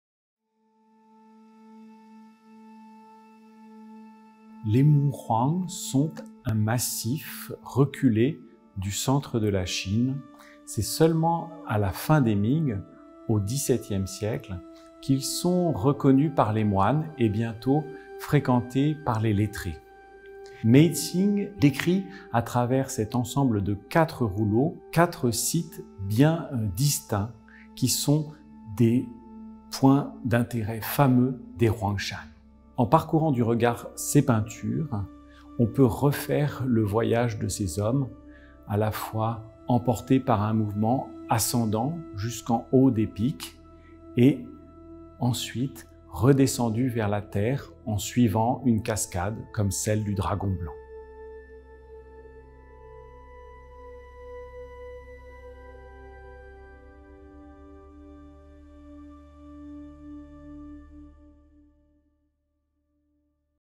Focus oeuvre : Mei Qing ( 1624-1697) Les Monts Huang, non daté | Musée Cernuschi
Exposition Peindre hors du monde, Moines et lettrés des dynasties Ming et Qing | Musée Cernuschi, musée d'art de l'Asie de la Ville de Paris
Collection Chih Lo Lou © Musée d'art de Hong Kong